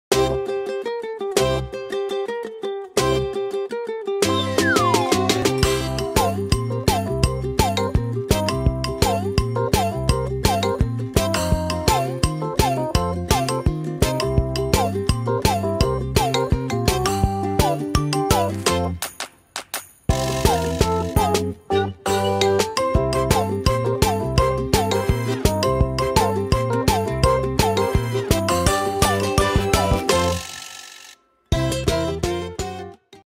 Полезный завтрак 2022/healthy breakfast
Полезный и быстрый завтрак, что готовится с утра из:
1. Голубика свежая;
2. Йогурт питьевой;
3. Блин готовый.
Сам продукт шикарен и нежный, а ещё составляет по параметрам витаминов и микроэлементов 100% вариант для организма. А именно смешиваем всё в нужных пропорциях и отдельной миске.

A healthy and quick breakfast that is prepared in the morning from:
1. Fresh blueberries;
2. Drinking yogurt;
3. Pancake is ready.
The product itself is gorgeous and tender, and it also makes up a 100% option for the body in terms of vitamins and microelements. Namely, we mix everything in the right proportions and in a separate bowl.